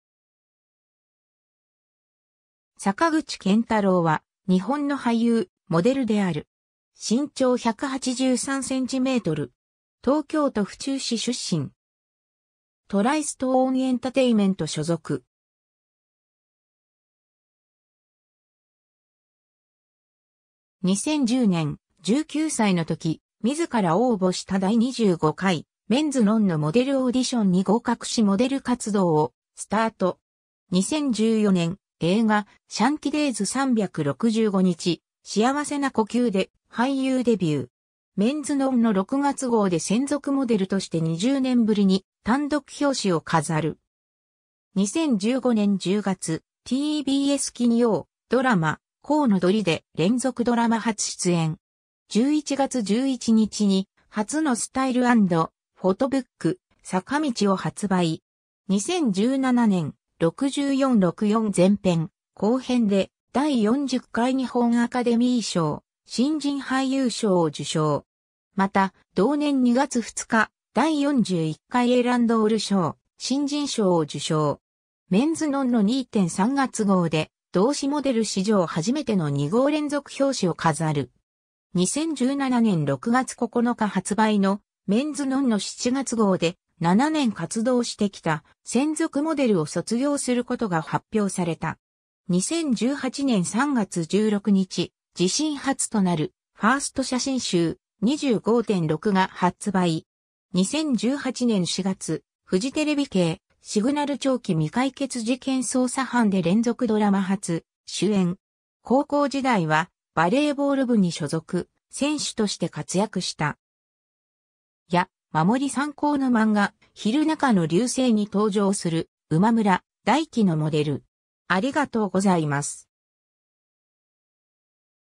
坂口健太郎
トライストーン・エンタテイメント
坂口 健太郎（さかぐち けんたろう、1991年〈平成3年〉7月11日 - ）は、日本の俳優、モデル。
身長183cm。
 東京都府中市出身。
トライストーン・エンタテイメント所属。
 2010年、19歳のとき、自ら応募した第25回『MEN'S NON-NO モデルオーディション』に合格しモデル活動をスタート。
 2014年、映画『シャンティ デイズ 365日、幸せな呼吸』で俳優デビュー。
メンズノンノ6月号で専属モデルとして20年ぶりに単独表紙を飾る。
 2015年10月、TBS金曜ドラマ『コウノドリ』で連続ドラマ初出演。
11月11日に初のスタイル&フォトブック『坂道』を発売。
 2017年、『64-ロクヨン- 前編/後編』で第40回日本アカデミー賞・新人俳優賞を受賞する。
また、同年2月2日、第41回エランドール賞・新人賞を受賞。
『MEN’S NON-NO』2・3月号で、同誌モデル史上初めての2号連続表紙を飾る。
 2017年6月9日発売の『MEN’S NON-NO』7月号で7年活動してきた専属モデルを卒業することが発表された。
 2018年3月16日、自身初となるファースト写真集『25.6』が発売された。
 2018年4月、フジテレビ系『シグナル 長期未解決事件捜査班』で連続ドラマ初主演を果たす。
 家族構成は母、姉。
なお、坂口の父は他界している。
 高校時代はバレーボール部に所属、選手として活躍した。
 やまもり三香の漫画『ひるなかの流星』に登場する馬村大輝のモデル。